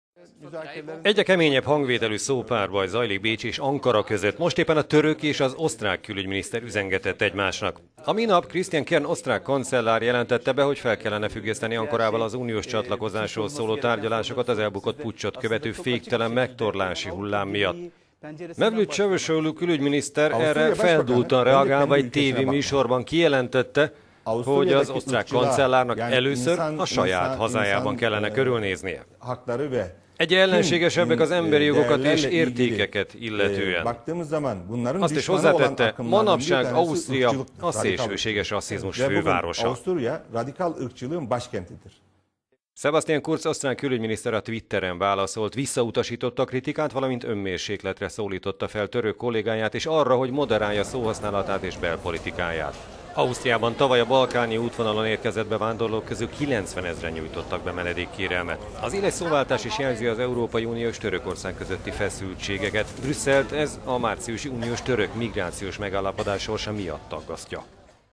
A török külügyminiszter rasszistának nevezte Ausztriát
Egyre keményebb hangvételű szópárbaj zajlik Bécs és Ankara között. Most éppen a török és az osztrák külügyminiszter üzengetett egymásnak.

A minap Christian Kern osztrák kancellár jelentette be: fel kellene függeszteni Ankarával az uniós csatlakozásról szóló tárgyalásokat az elbukott puccsot követő féktelen megtorlási hullám miatt.

Mevlüt Çavuşoğlu külügyminiszter erre feldúltan reagálva egy tv-műsorban kijelentette:

- Az osztrák kancellárnak először a saját hazájában kellene körülnéznie. Egyr…